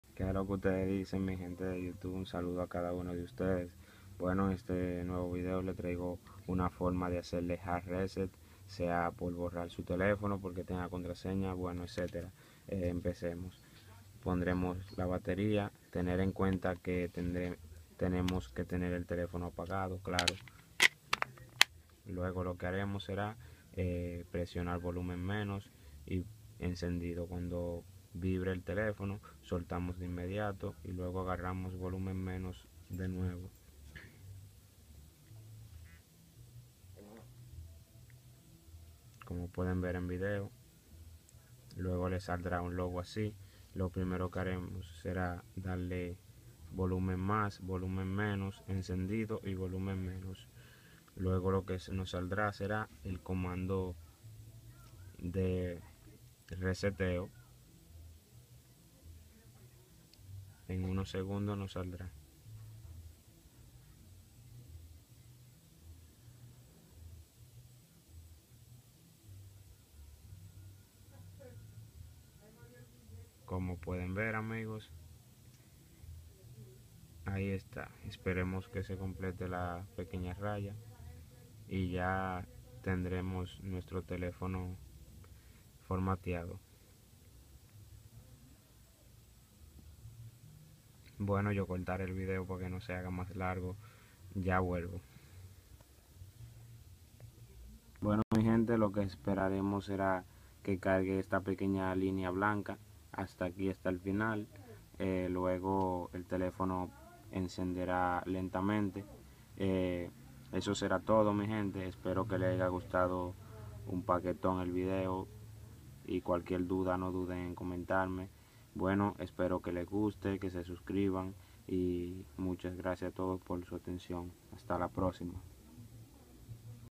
Hard reset Nokia Lumia 635
Saludos. Aquí les dejo este video para que aprendan como formatear este movil Nokia Lumia 635 , ya que a mi me dio mucha lucha para hacerlo anteriormente.